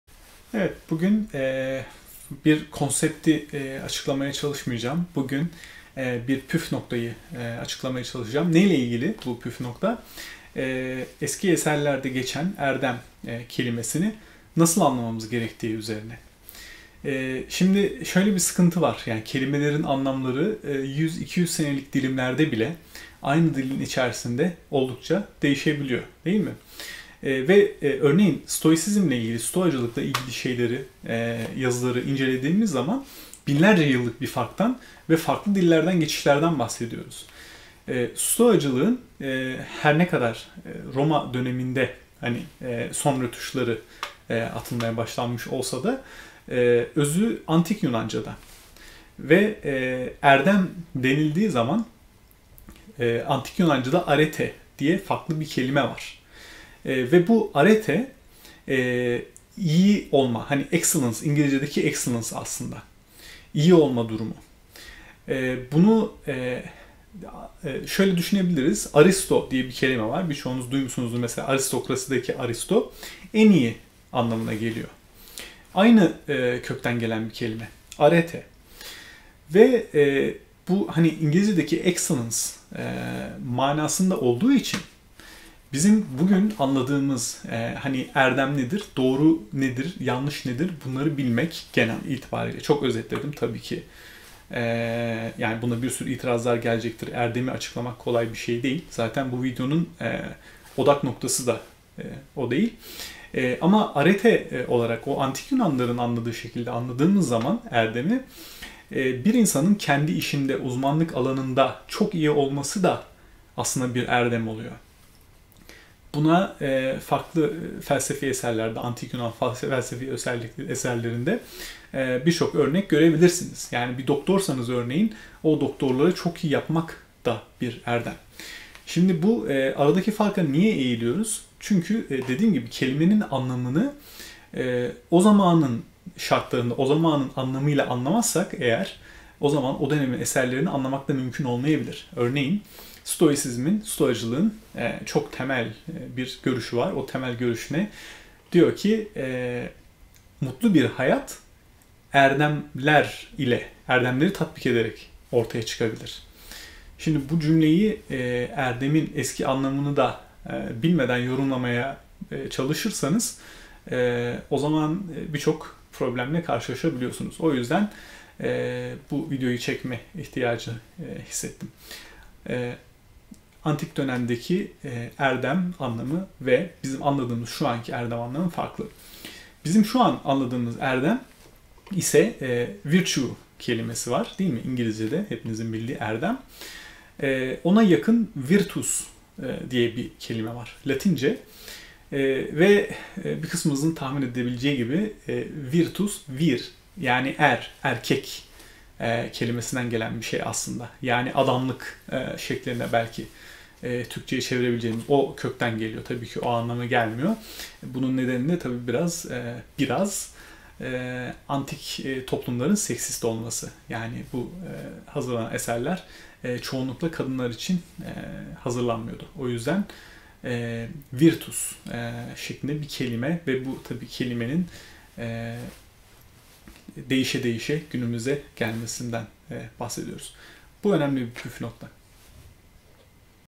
Felsefe Çalışma Notlarım - Erdem ve Arete
Erdem her zaman günümüzdeki anlamında değildi.
Bu anladığım kadarıyla eski eserlerin anlaşılmasında zaman zaman insanlarda kafa karışıklığına neden olabiliyor bazen.
Aslında düşününce arada yine aşırı bir fark yok, ama kelimenin ağırlık merkezi değişiyor, bu da sıkıntılara yol açabiliyor.